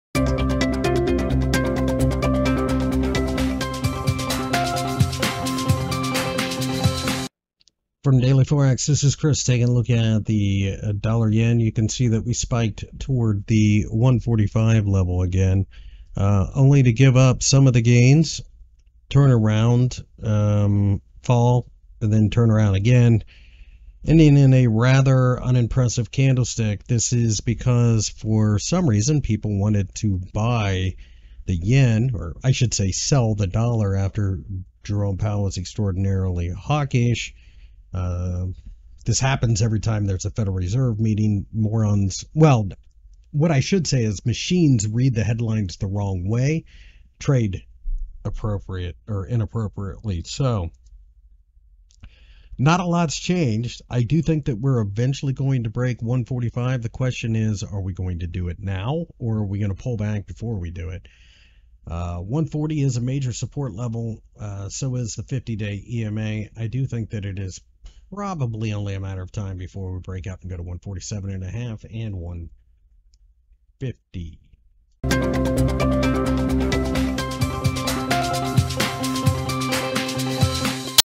USD/JPY Predictions: Who Will Win This Battle? | USD/JPY Forecast September 22, 2022 | DailyForex
DailyForex experts provide every day the most updated USD/JPY trading analysis.